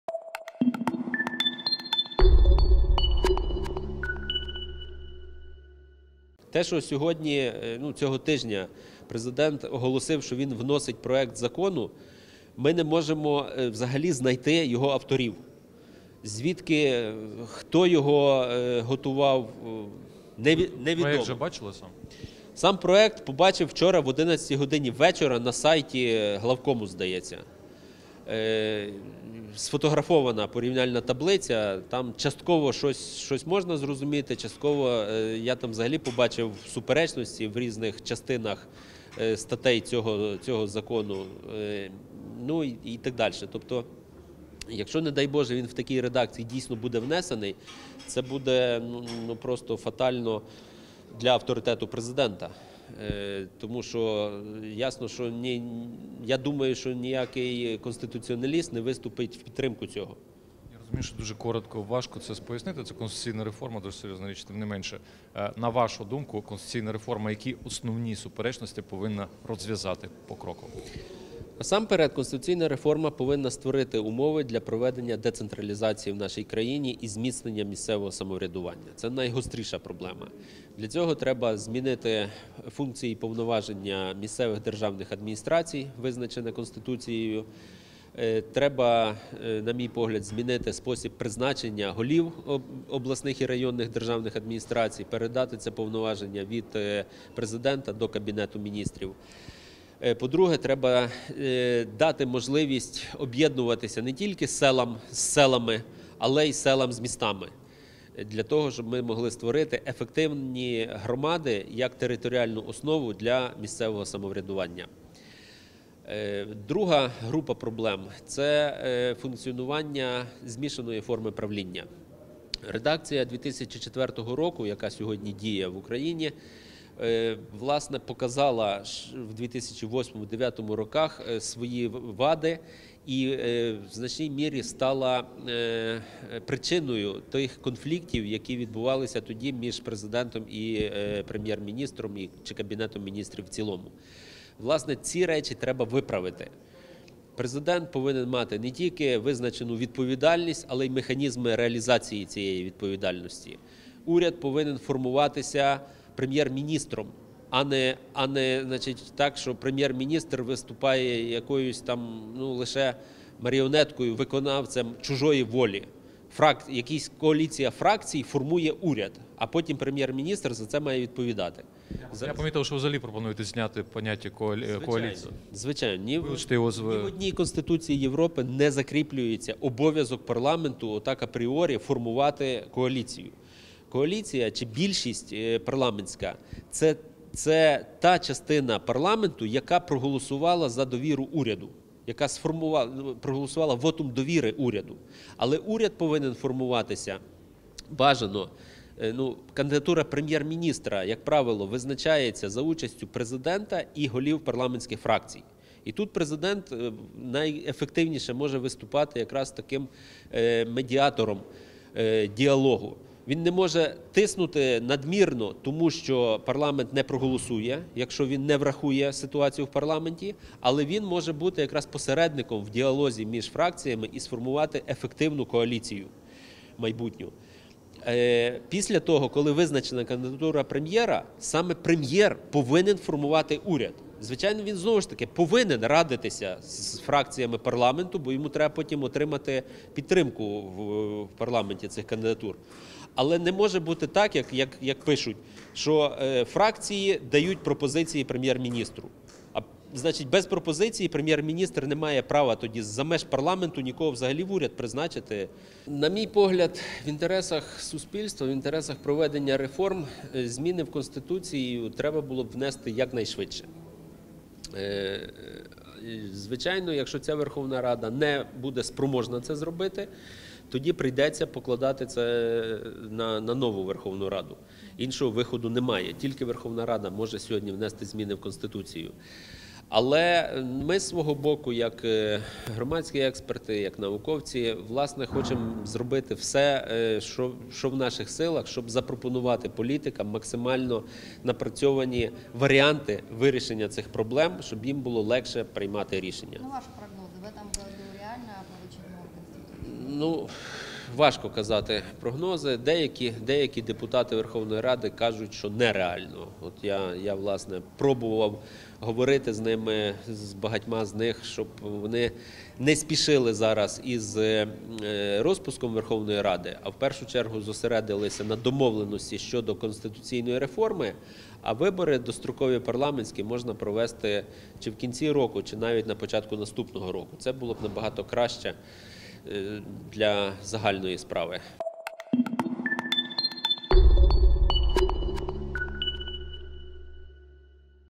Експерт: проект Конституції від Президента - поганий
Голова Центру політико-правових реформ Ігор Коліушко називає президентський проект змін до Конституції, який з'явився в медіа, сирим і непідготовленим. Експерт скаржиться на відсутність діалогу між парламентом, президентом та експертним середовищем у розробці збалансованого варіанту нової Конституції. Проект, підготовлений науковцями-конституціоналістами передбачає посилення фігури прем'єр-міністра, розмежування повноважень Кабміну і Президента, збільшення повноважень місцевих рад, усунення прокуратури від наглядових функцій, вилучення положень про коаліцію і багато інших новацій.